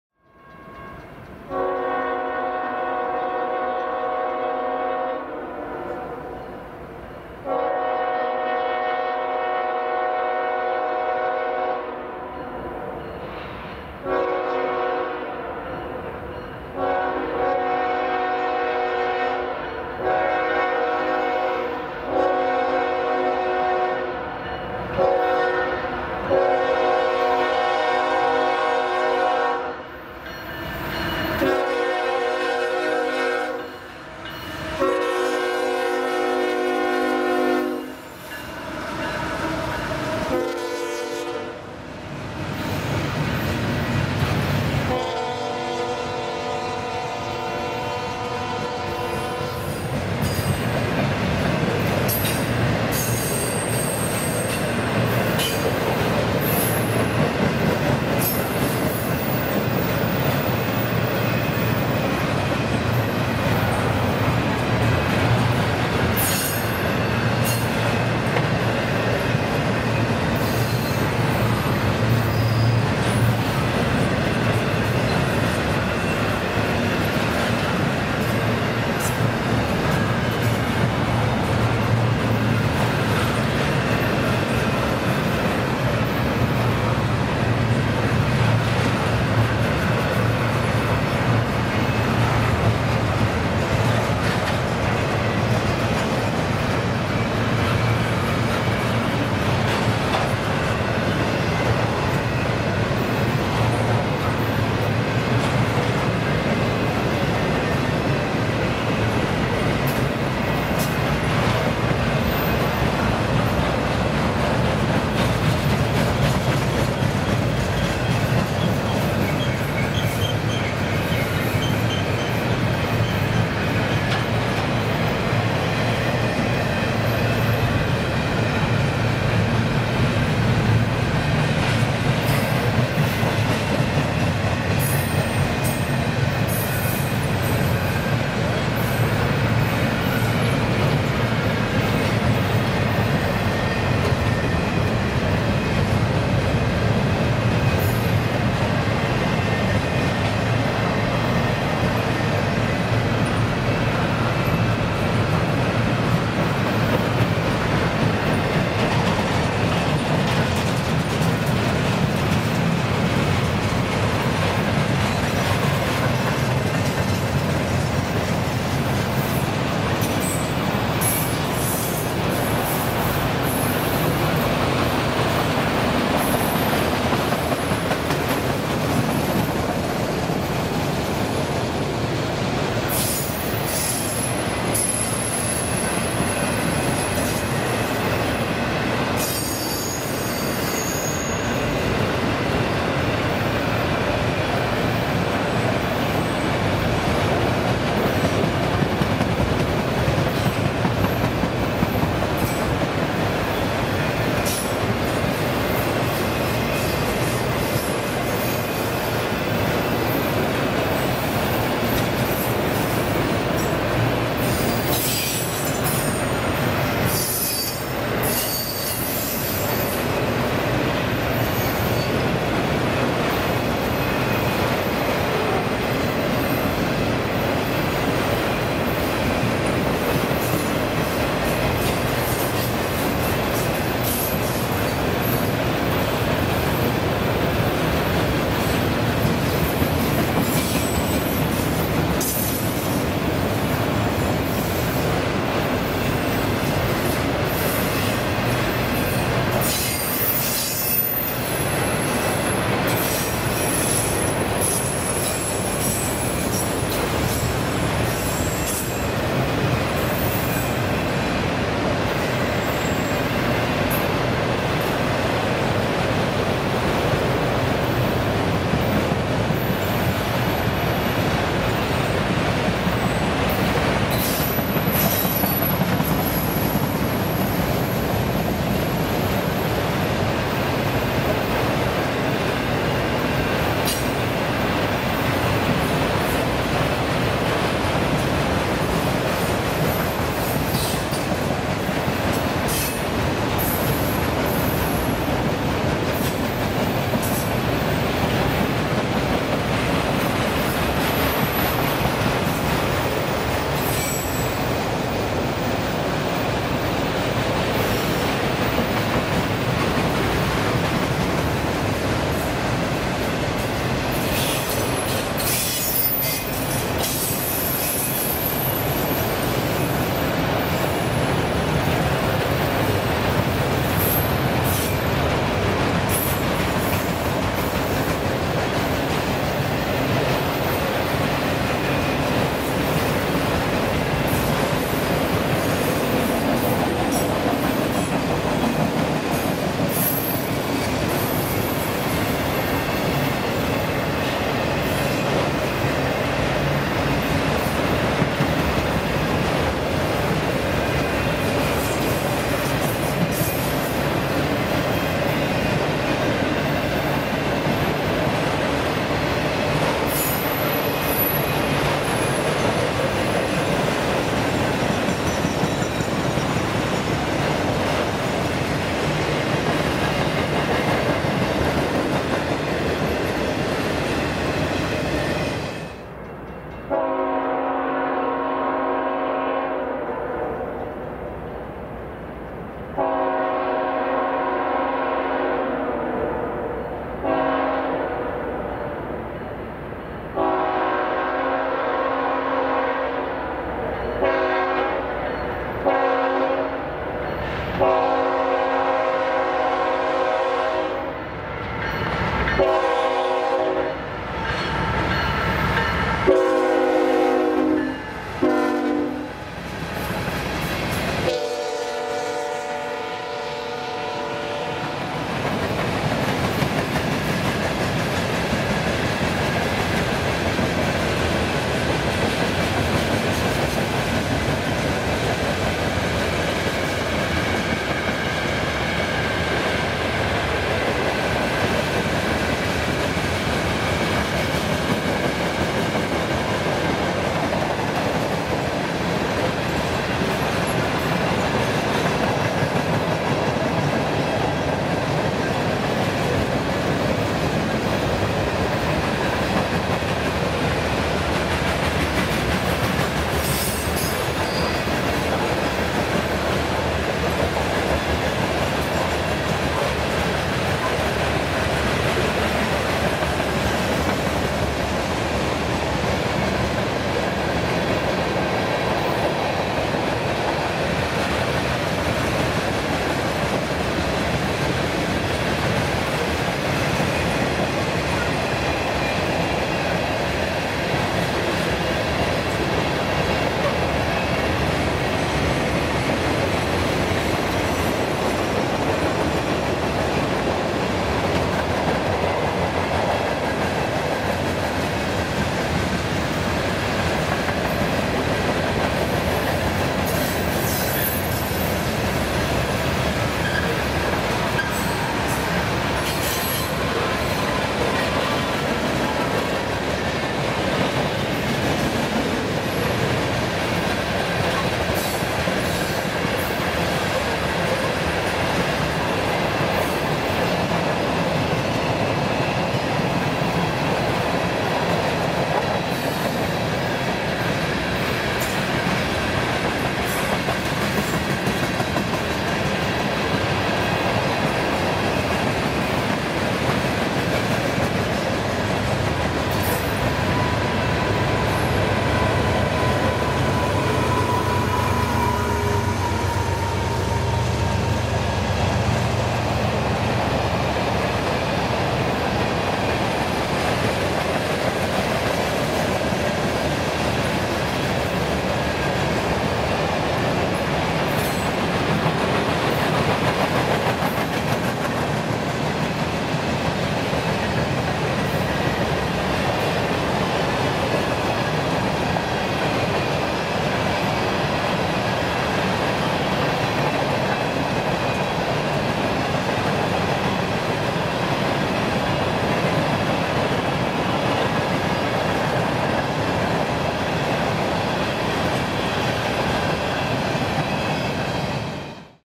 Two Long Freight Backs, One After Another, In A Quiet Georgia Town
Here we catch two CSX Freights rolling through Folkston, Georgia on Monday February 28th 2022. First up is Train Q261 heading to Jacksonville, Florida's Duval Ramp where it will drop off a healthy sized cut of Intermodal before proceeding west to Baldwin. Then up next is Train Q60328 heading to Winston Yard in Lakeland, Florida.